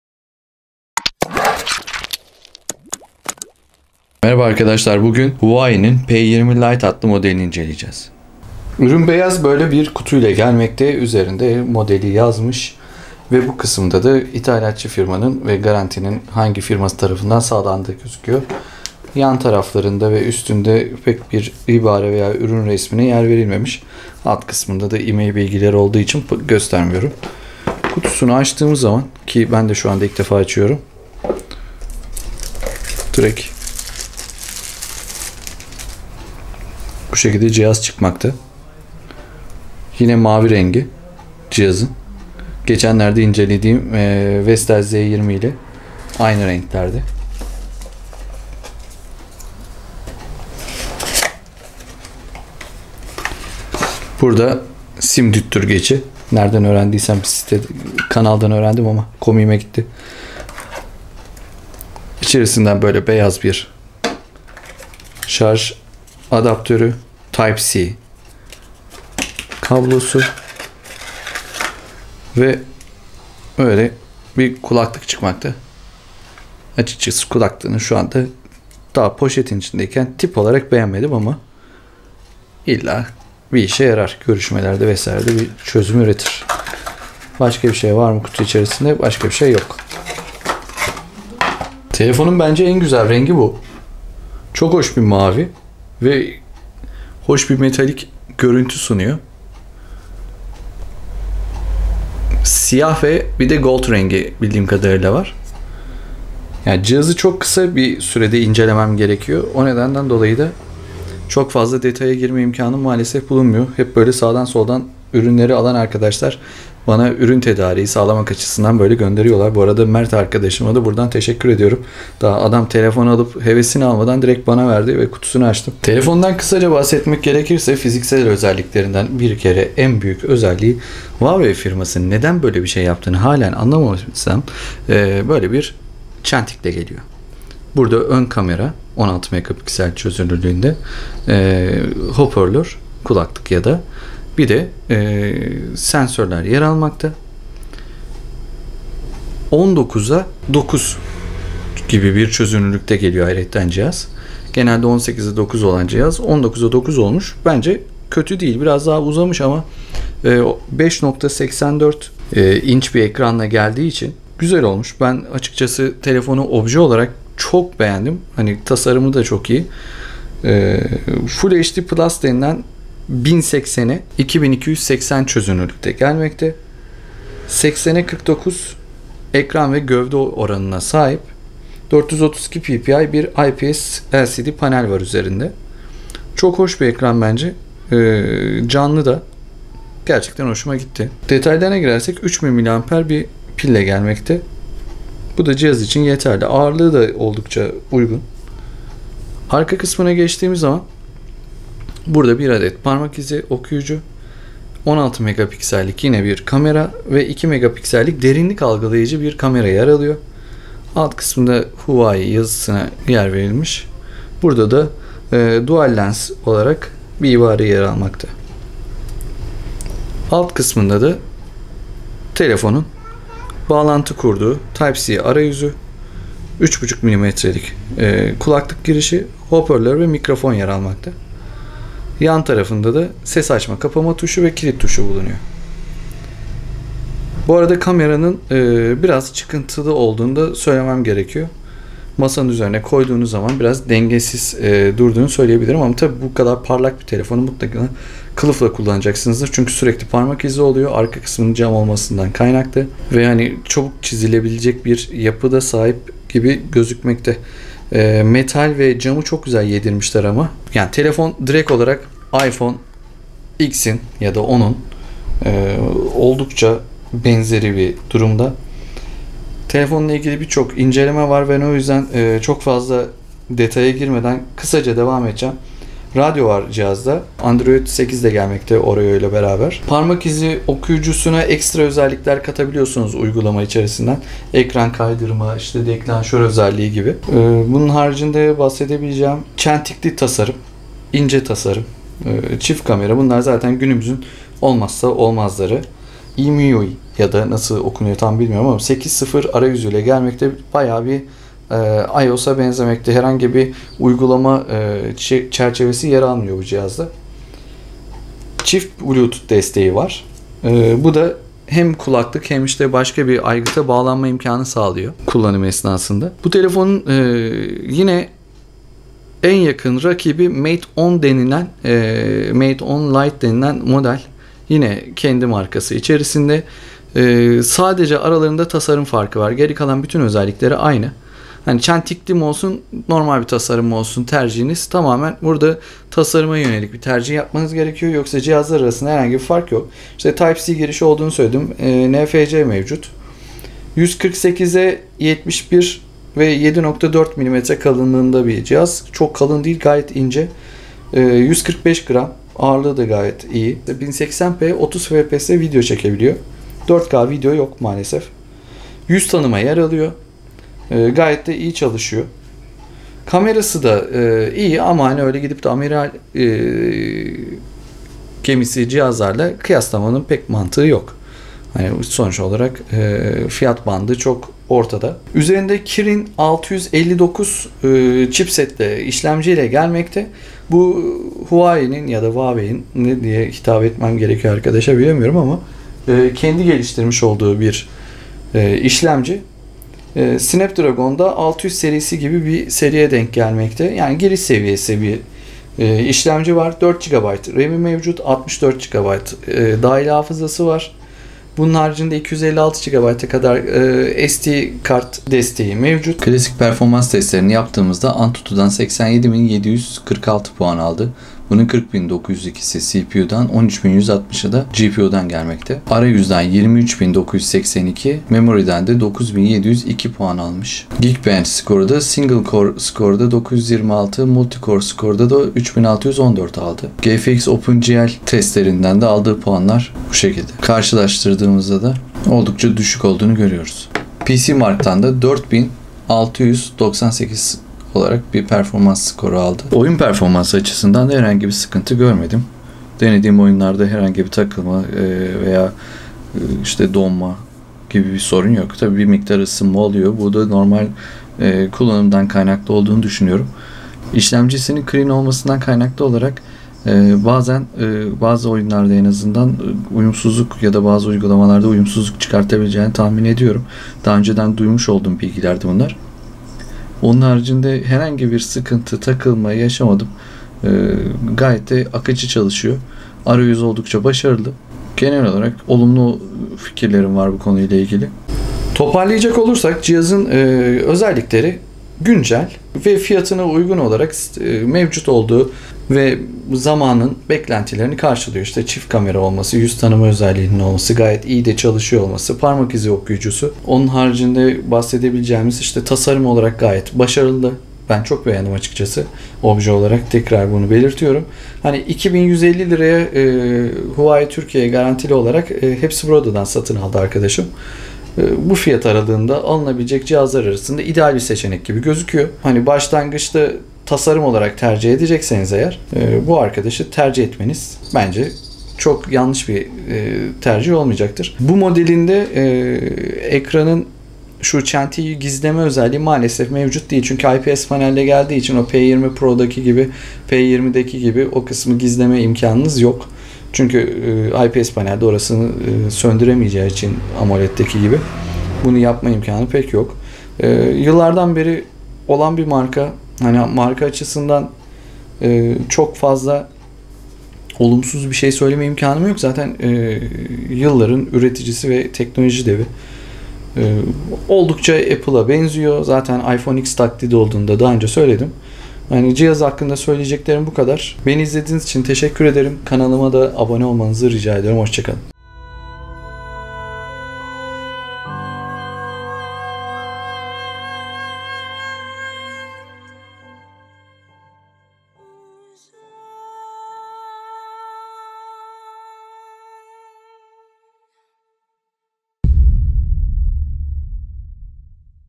Huawei P20 Lite İnceleme
Kameralar
Sony A7 Mark II

İşlemci Huawei Kirin 659 4 x Cortex-A53 2.36 GHz + 4 x Cortex-A53 1.7 GHz
4 GB RAM + 64 GB Depolama ve MicroSD
Ekran 5.84 inç 2280x1080
Kamera 16+2MP çift kamera ve 16 MP özçekim kamerası
USB-C
Parmak izi okuyucu
NFC
WiFi ac
7.4 mm 145 gram
3000 mAh
Android 8

Huawei P20 Lite inceleme - Fiyatını hak ediyor mu? Mate 10 Lite seçmek mantıklı mı?

P20 Pro'nun kardeşi Huawei P20 Lite kutusundan çıkıyor!